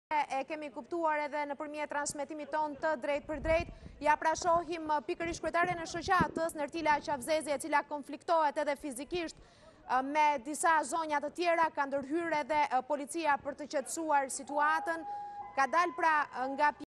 Nertila Qafzezi sulmon fizikisht të shkarkuarit nga Basha
Ora Neës përcjell momentet kur Nertila Qafëzezi sulmoi fizikisht grupin e të shkarkuarve nga bashkia të cilët rreth orës 13:00 u paraqitën para çadrave duke thënë se do u bashkohehin grevistëve.
Nga pamjet duket qartë që Nertila është tepër e irritura dhe pavarësisht se një burrë mundohet ta qetësojë dhe ta mbajë ajo vërsulet. Fillimisht gjuan me shkelm një burrë dhe më pas sulmon dhe më pas I turret një gruaje duke e shtyrë me forcë. Në këtë moment është dashur ndërhyrja e policisë për të tërhequr Nertilën e cila iu vërsul gruas me flokë të verdha duke tentuar ti kapë edhe flokët.